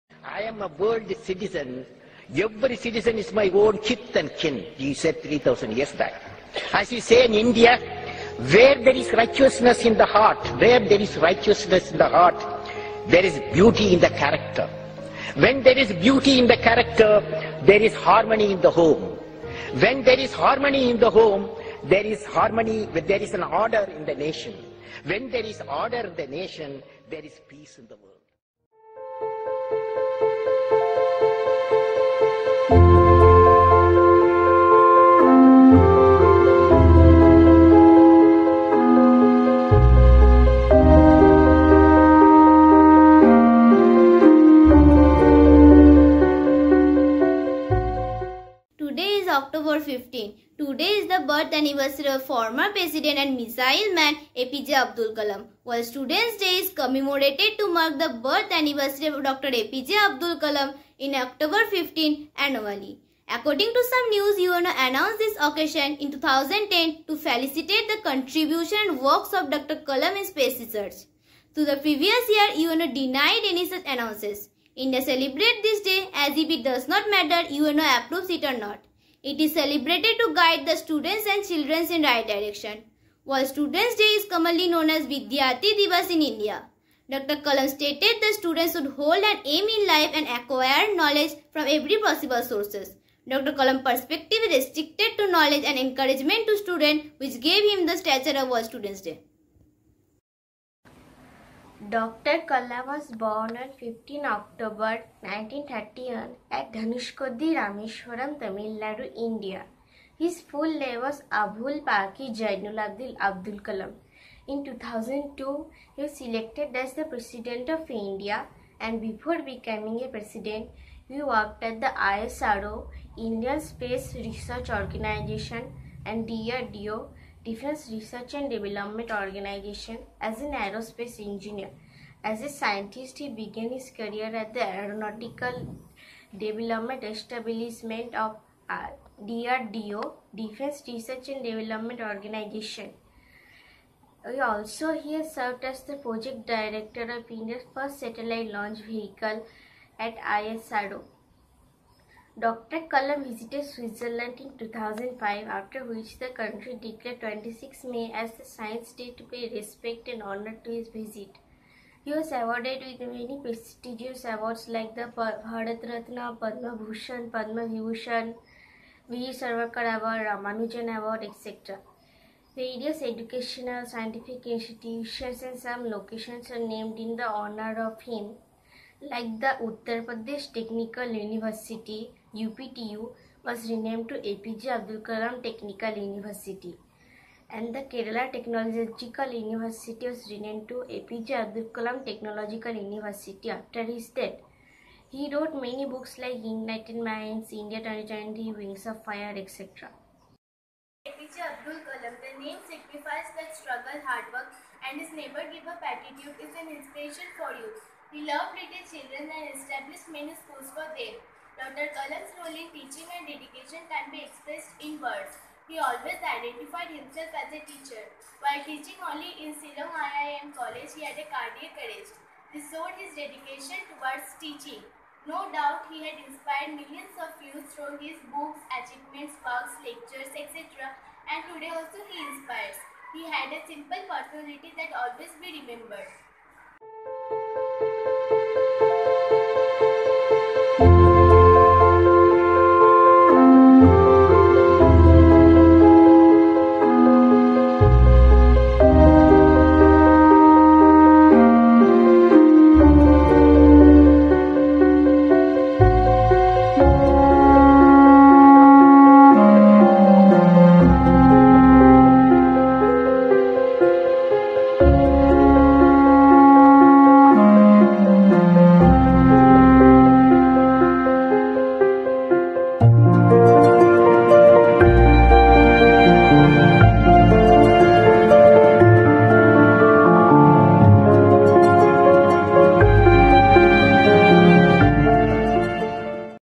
World Students' Day | Dr. A.P.J. Abdul Kalam | South End School
The 'Missile Man' of India Dr. A.P.J. Abdul Kalam's larger-than-life teachings continue to inspire students and skill aspirants all over the country. On this auspicious WORLD STUDENTS' DAY, let’s pay tribute to the personality who encouraged skill-based learning among students & motivated them to think out of the box.

This video is published by : South End School

School address : Paschim Bazar, Nalhati, Dist.- Birbhum, West Bengal, PIN - 731243